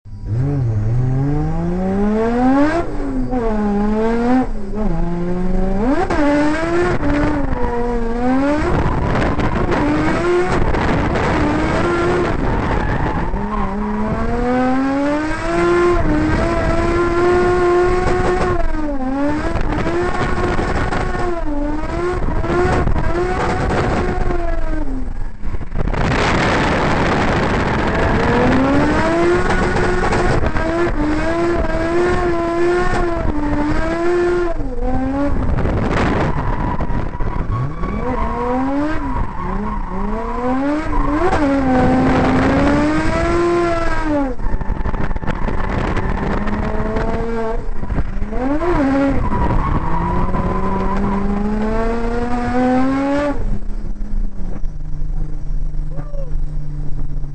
POWERCRUISE 20 drift demo car camera
another powercruise vid of me doing skids